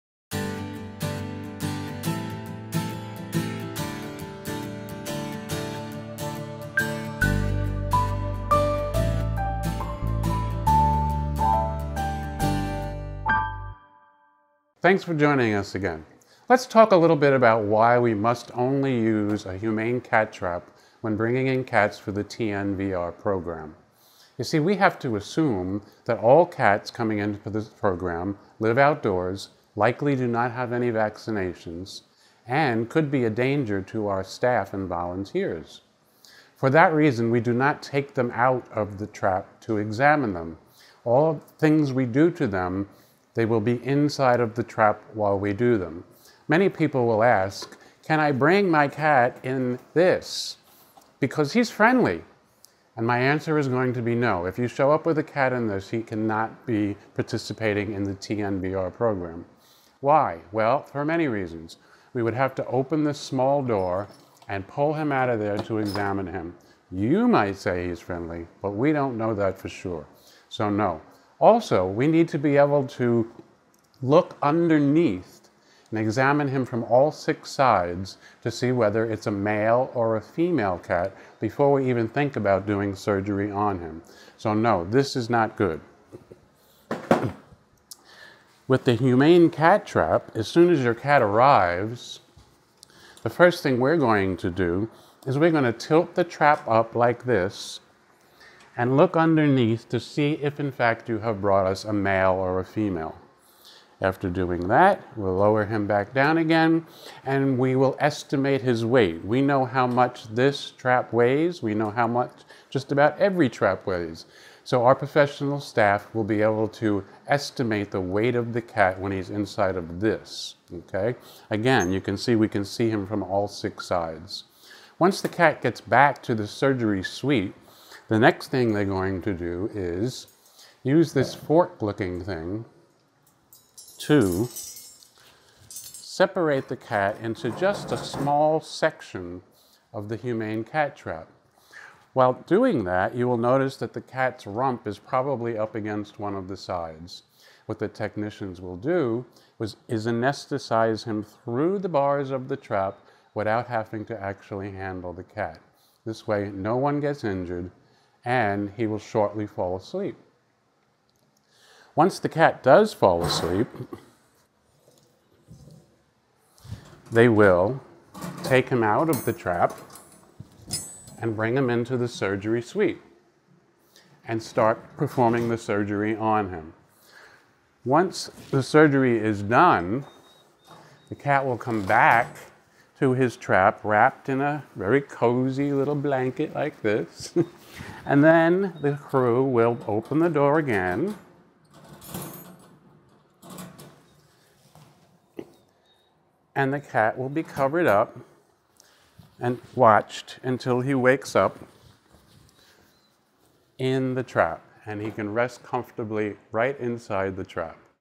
TNVR | Only in a Humane Trap
Seeing cats in your neighborhood? Learn about the Trap Neuter Vaccinate and Return to Field program at Peggy Adams Animal Rescue League in Palm Beach County Florida. You'll learn all about the TNVR process and how it helps cats in our community.
To learn more, visit PeggyAdams.org/TNVR